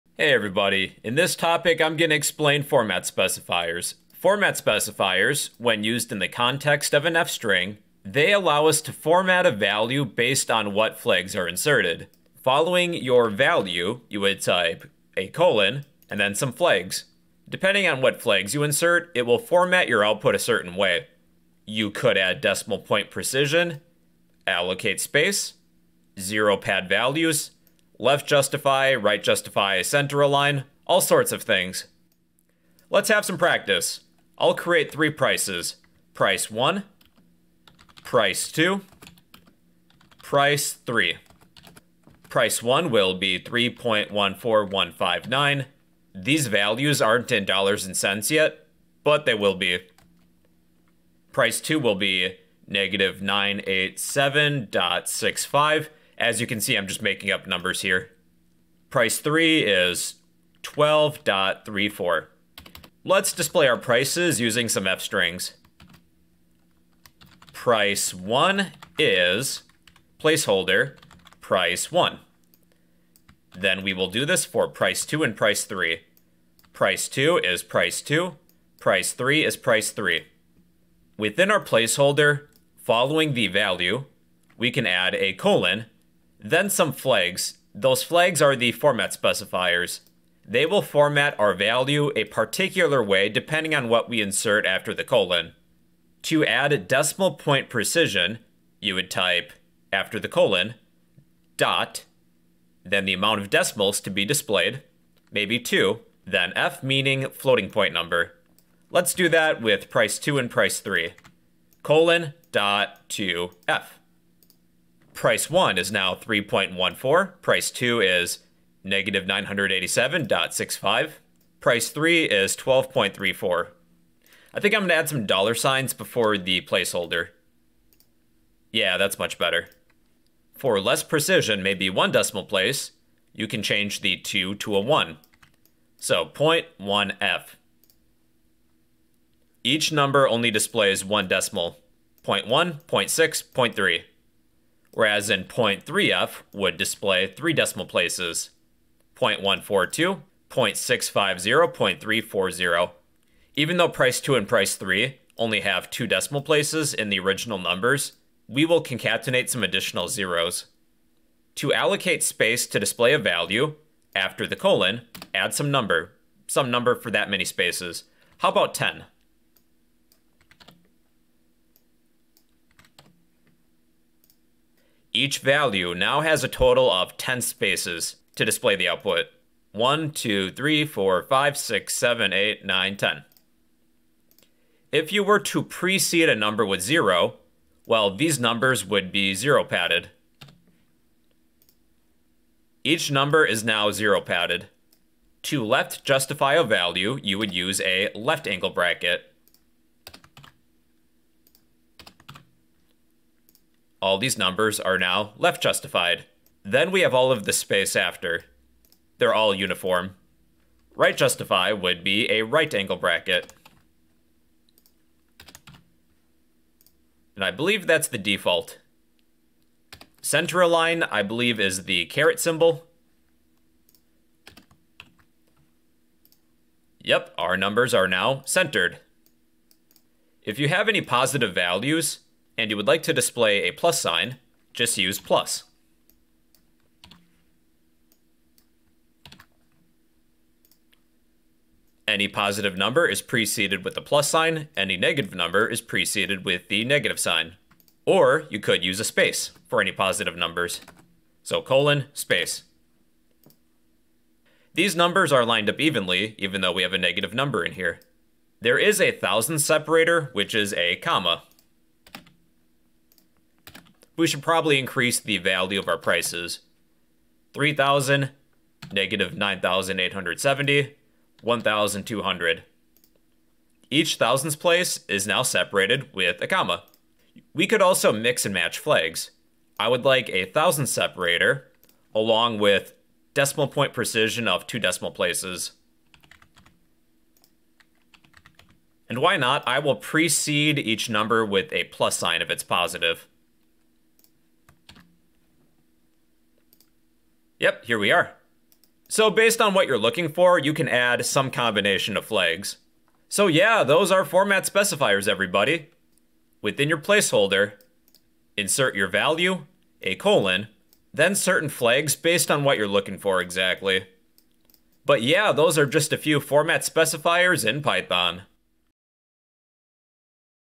Learn Python format specifiers in 5 minutes! 💬
# format specifiers = {:flags} format a value based on what flags are inserted

00:00:00 intro
00:01:35 decimal precision
00:02:41 padding
00:03:15 justify
00:04:18 comma separator
00:04:37 mix and match format specifiers
00:05:03 conclusion